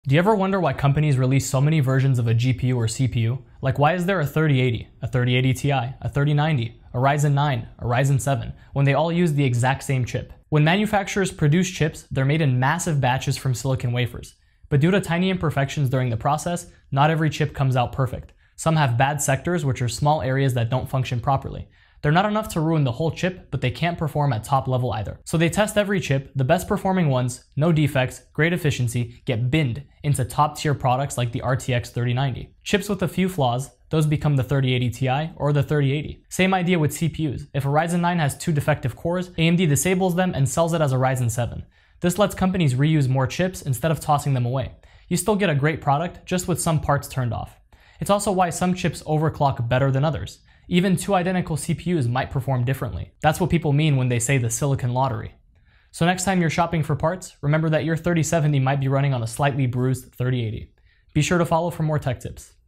Why are there so many CPU and GPU models?
Why are there so many versions of the same CPU or GPU? Ever noticed how a 3080, 3080 Ti, and 3090 all look similar but have different price tags? That’s binning. In this quick explainer, we break down how manufacturers sort chips based on performance, power efficiency, and defects. Turning one design into multiple models. It’s all about squeezing value from every chip off the assembly line.